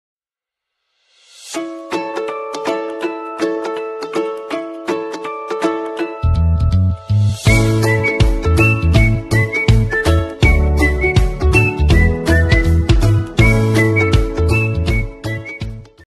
Cacto mandacaru/fases/Cunha.
Olha que lindo esse cactos ,delels até abrir a flor mandacaru e da cunha esse cactos.